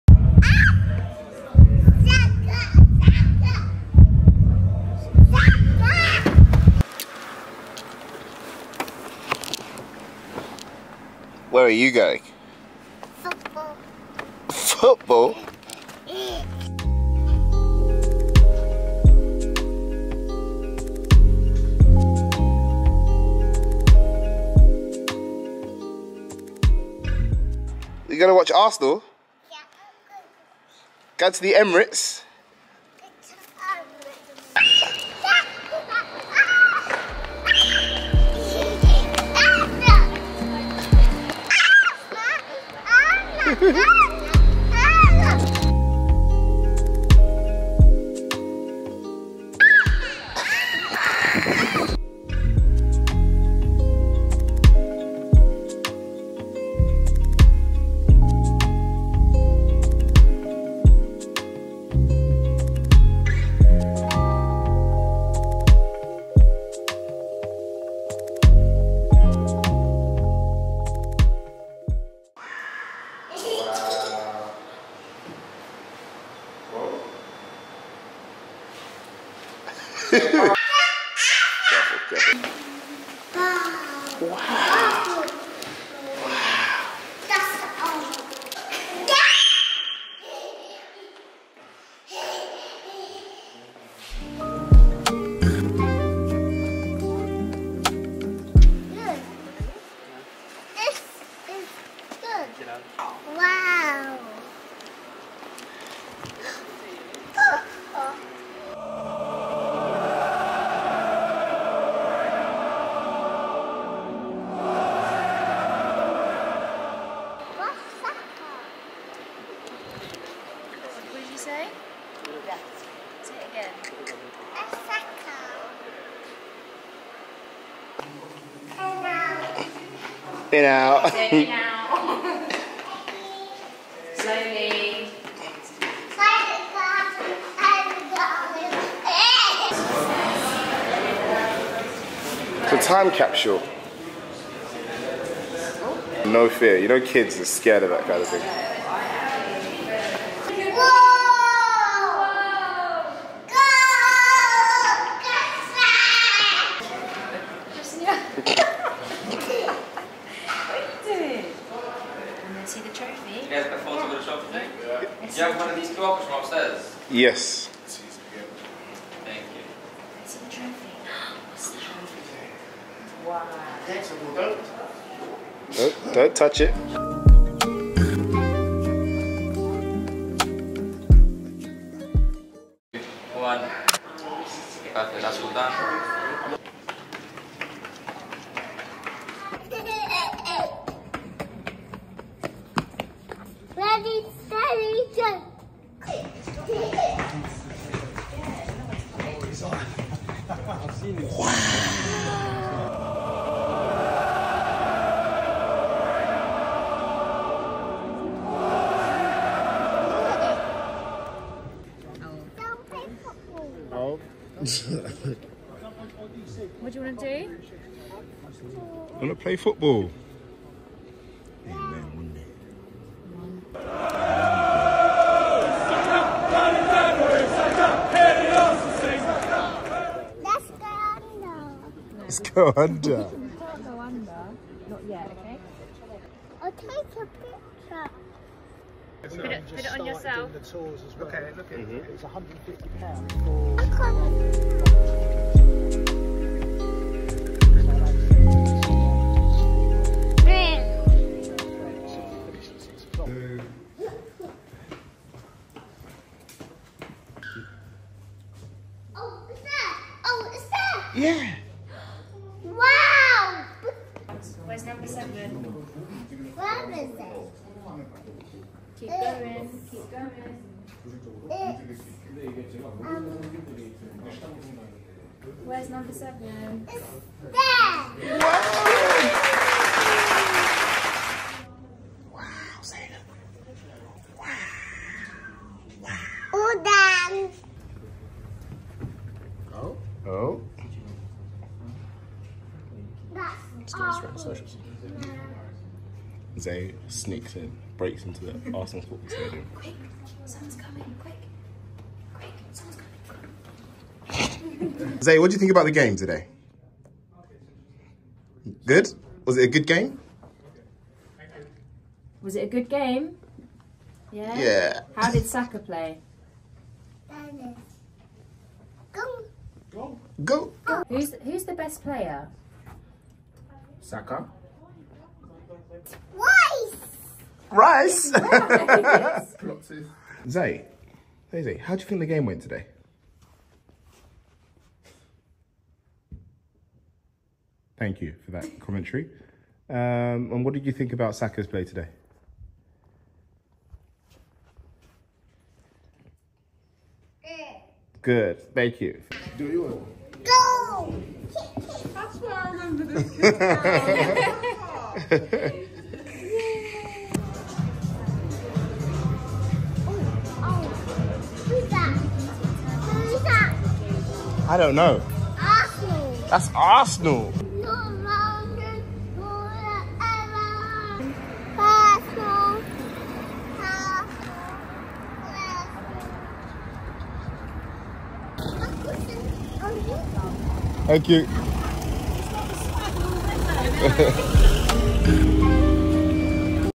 Zai goes to the Emirates stadium!
Hey guys, Welcome to Zai Zai's Youtube channel!
As you know Zai is a massive football fan... ARSENAL (Saka GOAL!) Here is a video of Zai's going to Arsenal stadium!